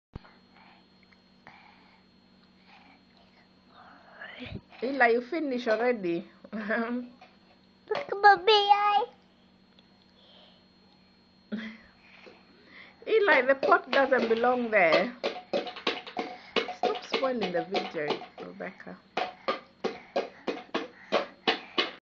Shapes...Eli sorting shapes out
Eli and Rebecca playing together